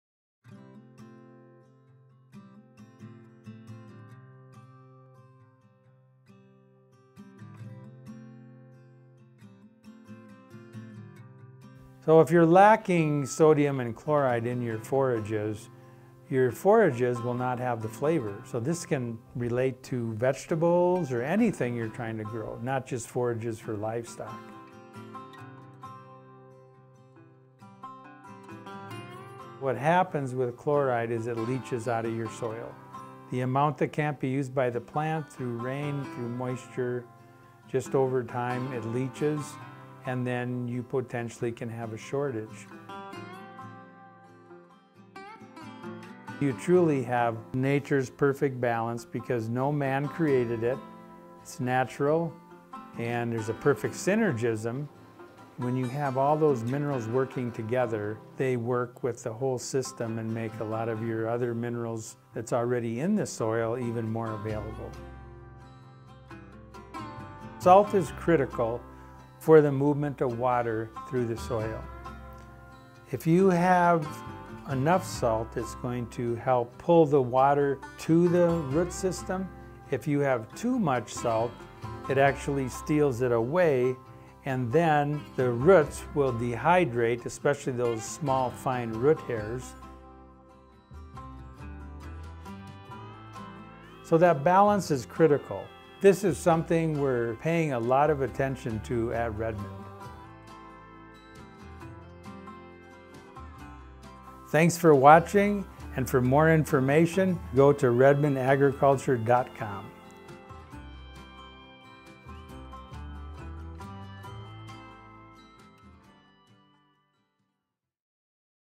How can Redmond Salt benefit your soil and plants?
The three key points I have learned are:
Chloride is listed as one of the top 16 essential nutrients by soil scientists and it is critical for photosynthesis which is the conversion of energy from the sun to food for plants. Redmond salt contains over 50% chloride along with sodium and over 60 other highly available elements.
Chloride is a negative charged ion called an anion. Anions leach out of the soil just like nitrogen, sulfur, and boron and should be replenished on a yearly basis as well.  Where there is average to above average moisture chloride leaches and when looking at soluble salt, sodium and salt levels on soil tests and sodium and salt levels on tissue tests and forage tests I usually see them very low to low. If salt levels are deficient in a plant, the plant will lack flavor just like in the food we eat.
Salt is critical for the movement of water through the soil to the root system. It draws water to the root system. Only when it is too high will it steal the water from the roots causing dehydration which is called root burning.